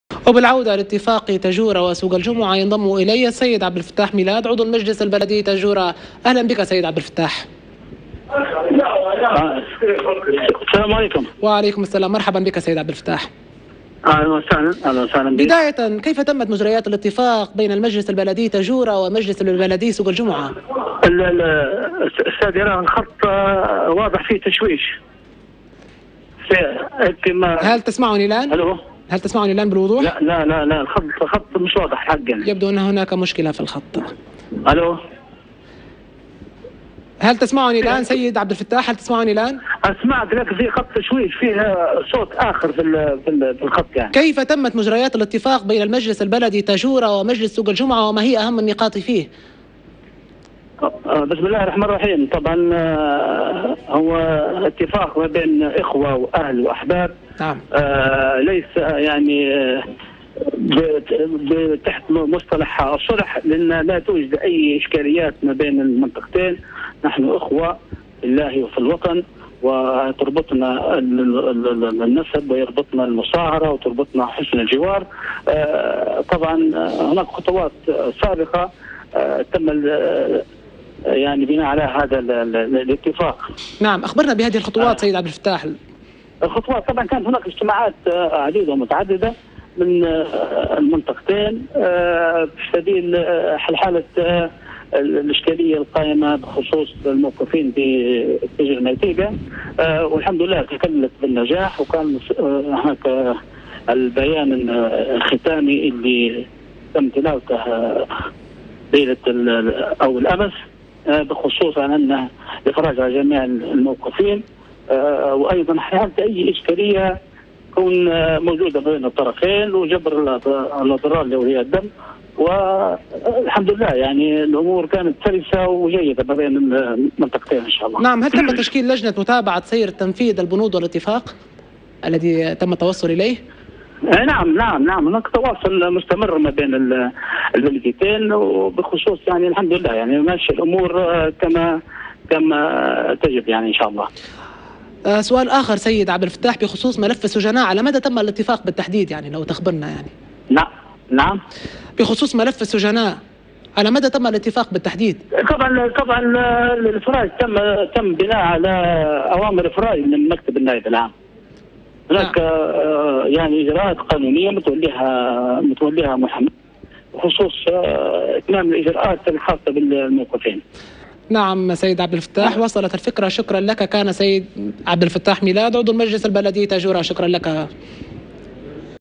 كيف تم الاتفاق بين المجلس البلدي تاجوراء والمجلس البلدي سوق الجمعة؟ تابع مداخلة أ.عبدالفتاح بن ميلاد
كيف تم الاتفاق بين المجلس البلدي تاجوراء والمجلس البلدي سوق الجمعة؟ وما أهم ما تم الاتفاق عليه؟  تابع مداخلة الأستاذ عبد الفتاح بن ميلاد عضو المجلس البلدي تاجوراء | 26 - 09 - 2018

ــــــــــــــــــــــــــــــــــــــــــــــــــــــــــــــــــــــــــ
ــــــــــــــــــــــــــــــــــــــــــــــــــــــــــــــــــــــــــ
يمكنكم تحميل تطبيقنا TANASUH TV على الهواتف الذكية: